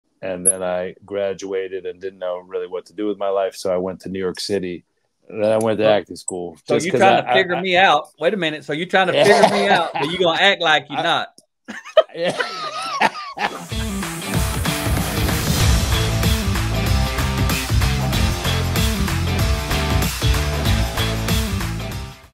So, you're trying to figure me out?
Watch a short clip from our interview with Matthew McGrath - the full interview releases on 04/19/24.